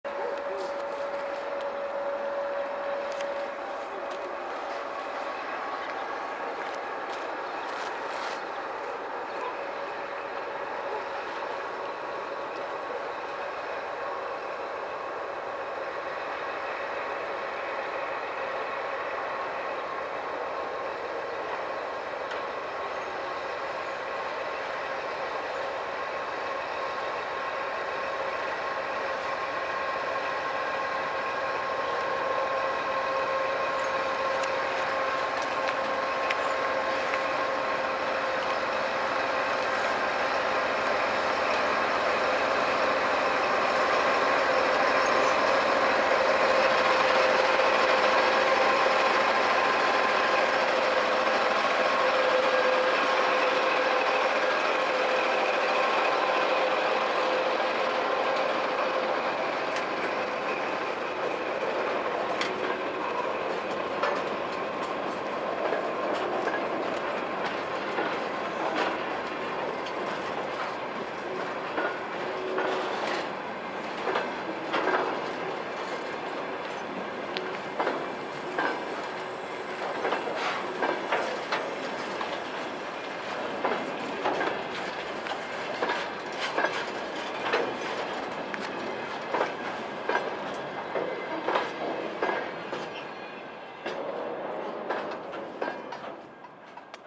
ТГМ4А-1795 с грузовым составом с ТЭЦ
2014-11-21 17:16 [preview image by Ismailov Bohdan]

Тепловоз ТГМ4А-1795 с грузовым составом
Подъездной путь черниговской ТЭЦ, станция Чернигов, Юго-Западная железная дорога, Украина

Diesel locomotive TGM4A-1795 with freight train
Chernihiv heating power station track, Chernihiv station, South-Western railroad, Ukraine

Тепловоз ТГМ4А-1795 з вантажним поїздом
Під'їздна колія чернігівської ТЕЦ, станція Чернігів, Південно-Західна залізниця, Україна